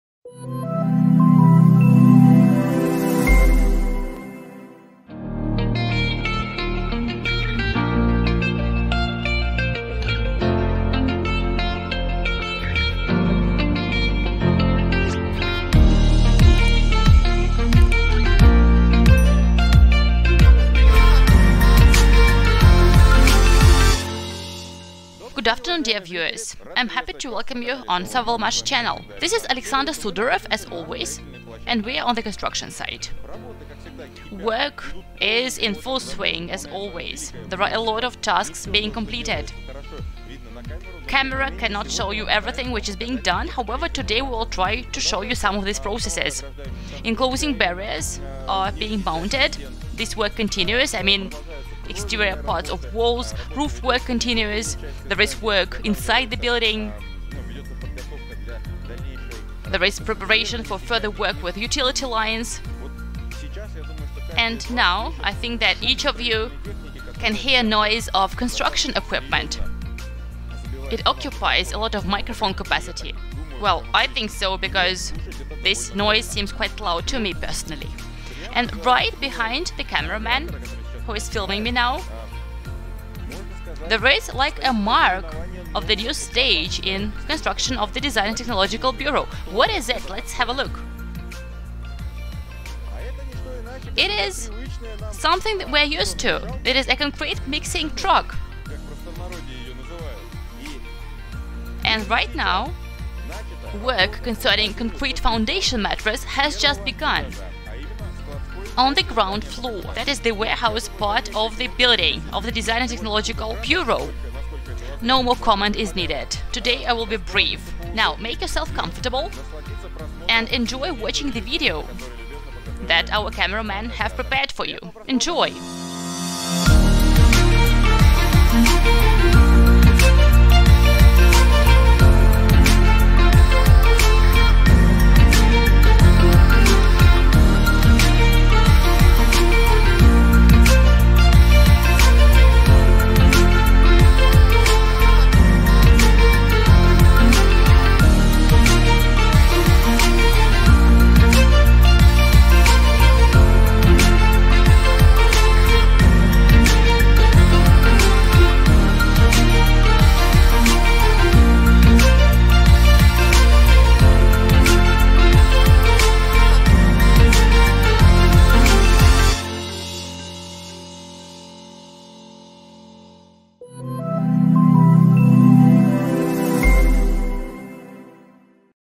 New stage in the D&E construction: concrete construction on the 1st floor of the building
The work is in full swing at the construction site where the "Sovelmash" design and engineering department is being built. The installation of the building envelope, namely the outer part of the walls, is underway, the roofing work is in progress, too. Inside the building, the preparations for further work with the engineering utilities are in progress.

New stage in the D&E construction has started: concrete construction on the 1st floor in the warehouse part of the building.

Watch the new video, add your comments and follow our news!